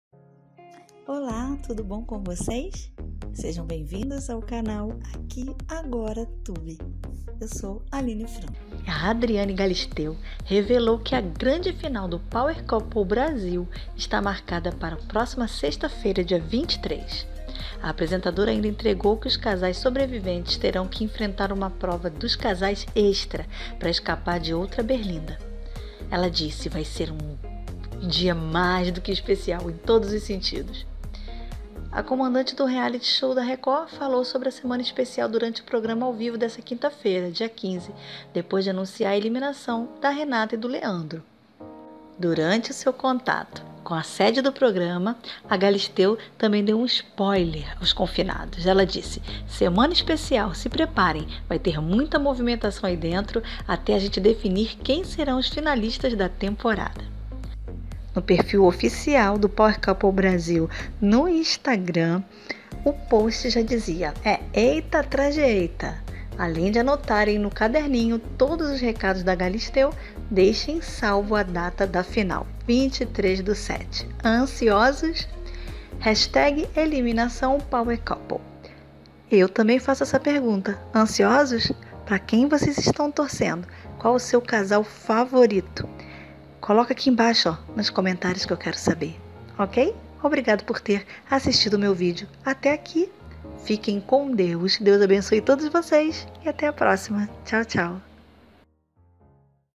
🔴 Que data termina o Power Couple ❓ Adriane Galisteu revelou!
A apresentadora Adriane Galisteu deu um spoiler para os espectadores sobre o final do programa.

Aqui você encontrará notícias sobre celebridades, programas de tv e principalmente sobre artistas e programação da Record TV.